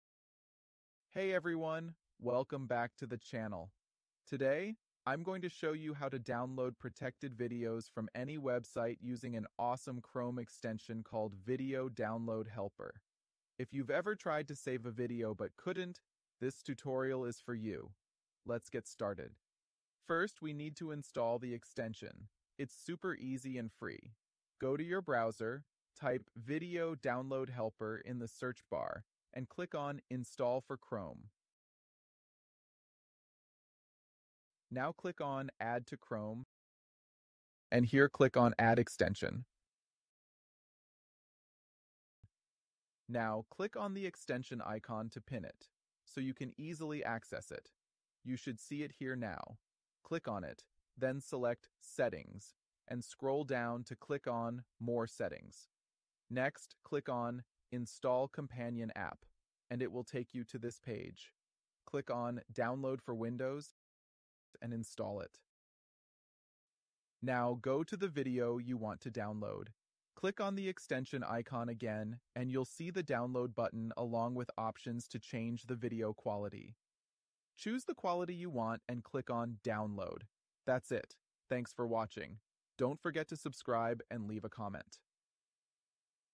HOW TO DOWNLOAD PROTECTED VIDEOS FROM ANY WEBSITE 2024
In this video, I’ll show you step-by-step how to download protected videos from any website, whether they are encrypted, DRM-protected, or embedded. Many websites restrict video downloads, but with the right tools and methods, you can easily bypass these protections. I’ll cover different techniques to save videos from streaming sites, premium content platforms, and more. Whether you need a browser extension, software, or an online downloader, I’ve got you covered! Make sure to watch till the end for the easiest and most efficient methods.